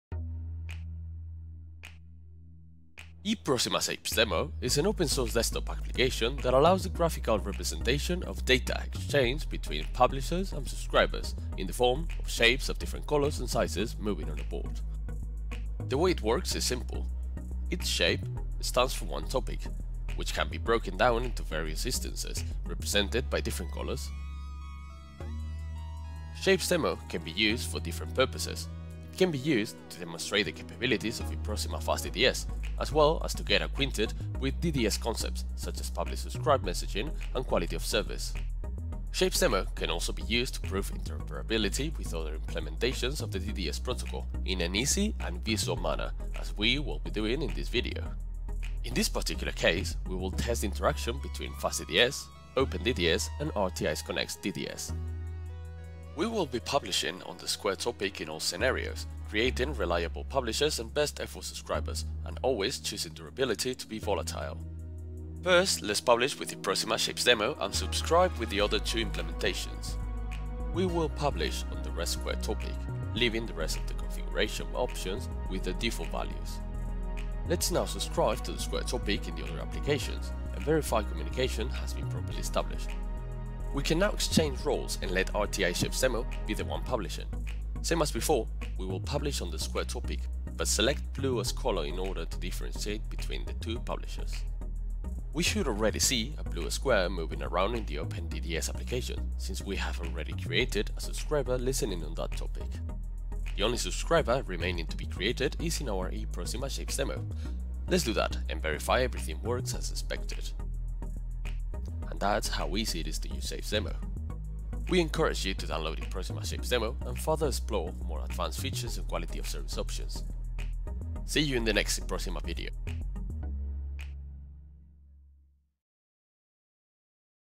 eProsima Shapes Demo with Fast DDS
eProsima Shapes Demo is an open source desktop application that enables the graphical representation of Publisher and Subscribers of eProsima Fast DDS.

eProsima Fast DDS is the most complete open source DDS implementation. This high performance  communicating middleware shares data in distributed systems using a decoupled model based on Publishers, Subscribers and Data Topics.

eProsima Fast DDS is compliant with the OMG standard DDS 1.4 and RTPS 2.2, and interoperable with the main implementations available, such as: RTI Connext DDS, OpenDDS.

This video demonstrates how to use eProsima Shapes Demo in order to:
- Demonstrate the capabilities of eProsima Fast DDS
- Prove interoperability with other DDS implementations